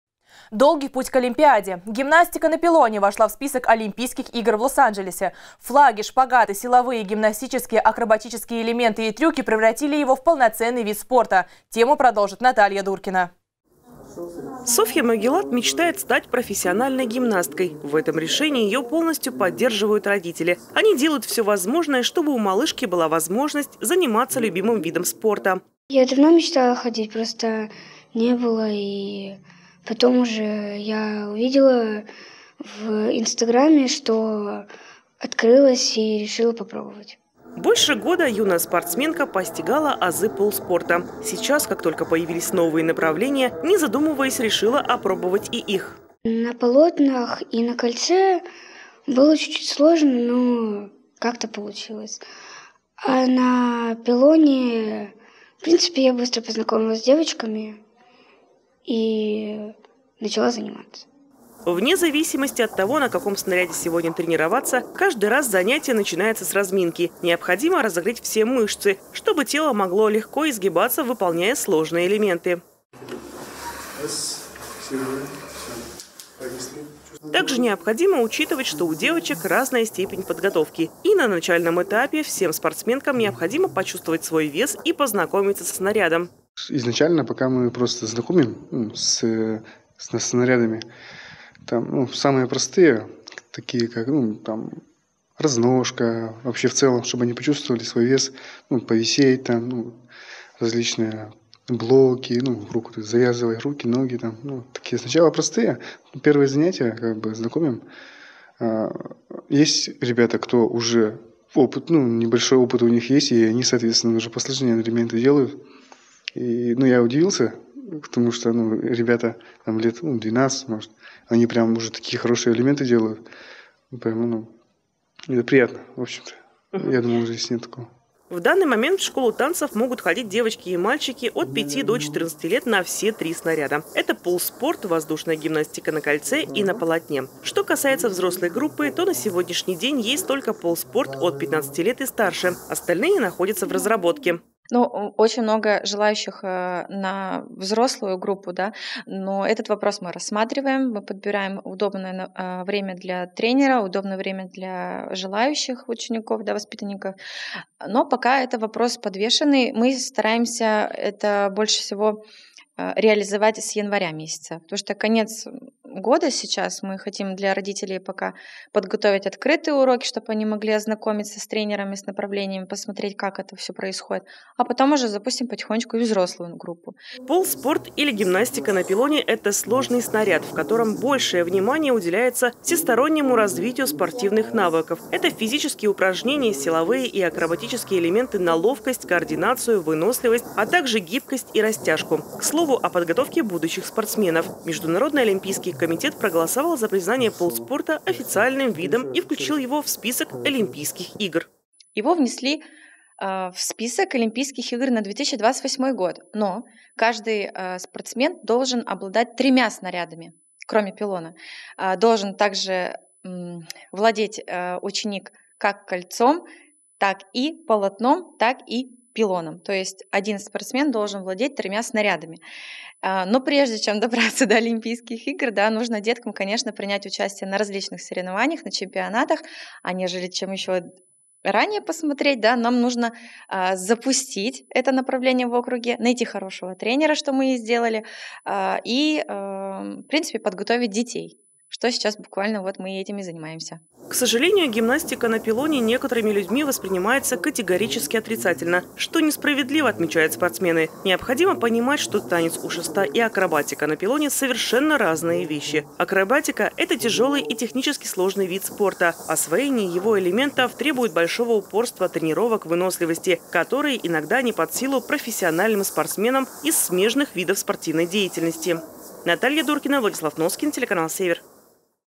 ПОДГОТОВКА ОЛИМПИЙСКИХ ЧЕМПИОНОВ
Долгий путь к олимпиаде. Гимнастика на пилоне вошла в список Олимпийских игр в Лос-Анджелесе. Флаги, шпагаты, силовые, гимнастические, акробатические элементы и трюки превратили его в полноценный вид спорта.